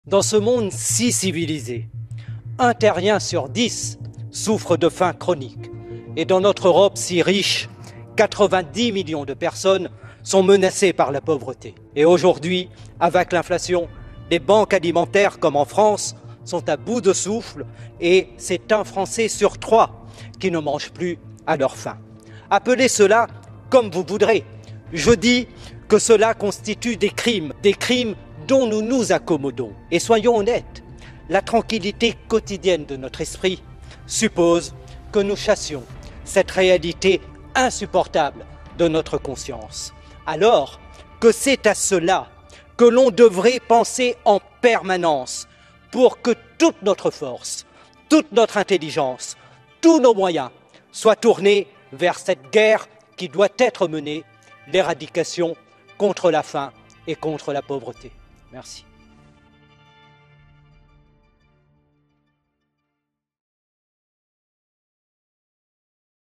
🔴 UN FRANÇAIS SUR 3 NE MANGE PAS À SA FAIM !
Notre priorité devrait être l'éradication de la faim et de la pauvreté.

Discours de Younous Omarjee, Député LaFranceInsoumise au parlementeuropéen.

➡️ Likez ma page Facebook : Younous Omarjee